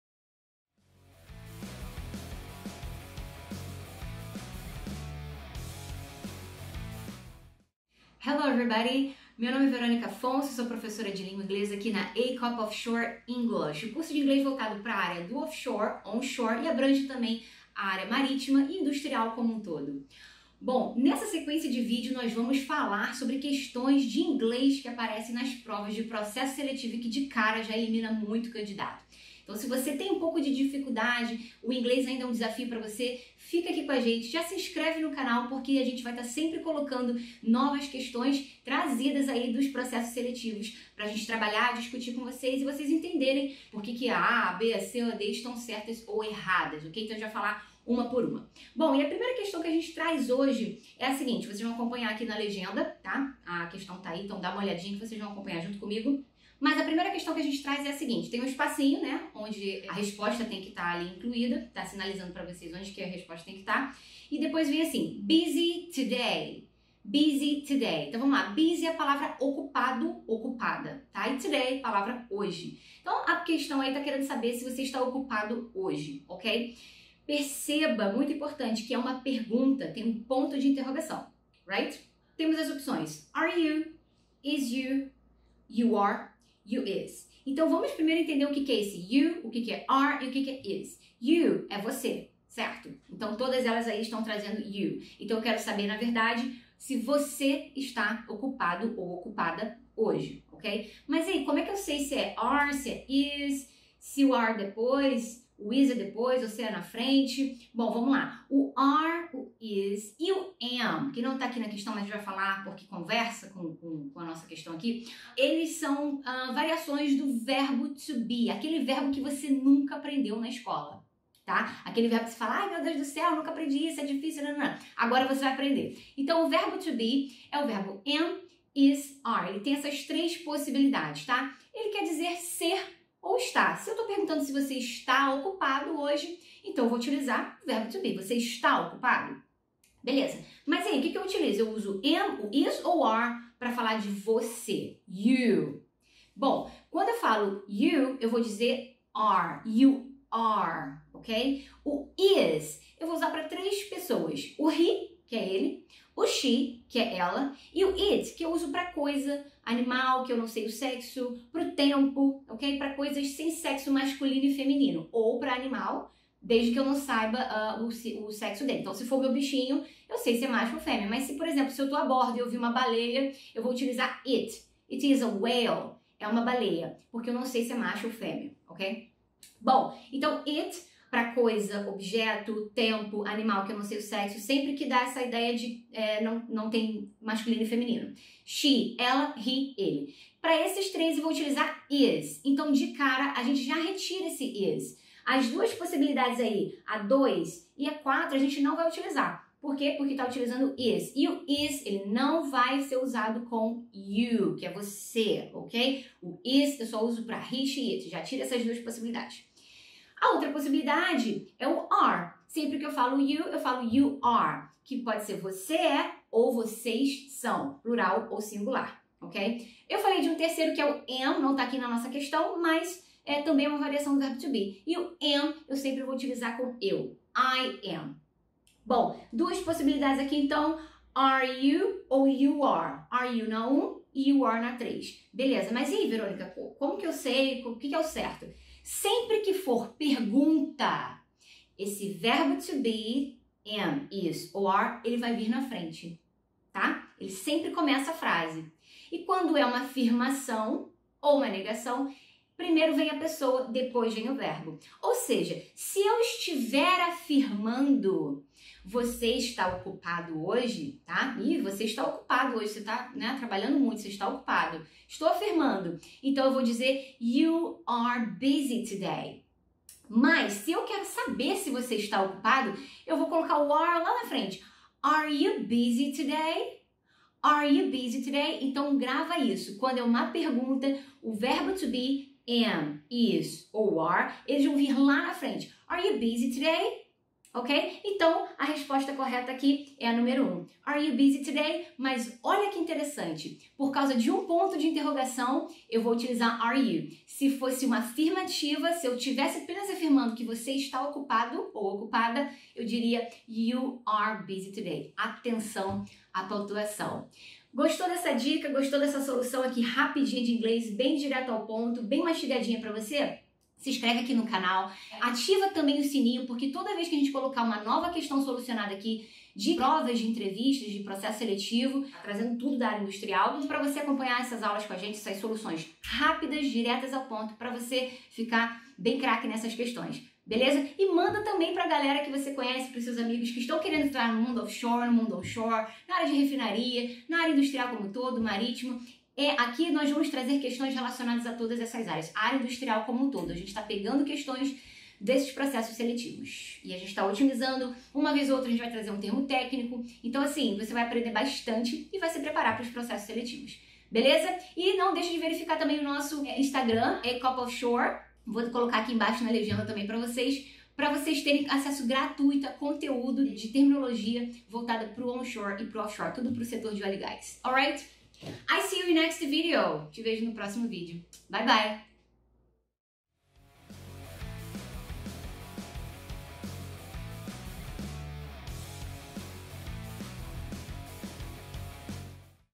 Questões de Processos Seletivos em Inglês de Empresas Offshore
Questões de processos seletivos em inglês passados pelos nossos alunos.
ECOP mais uma vez facilitando sua vida.

Não perca essa chance de estar por dentro das provas dos processos seletivos de empresas offshore.

Venha conosco!
Liberte-se!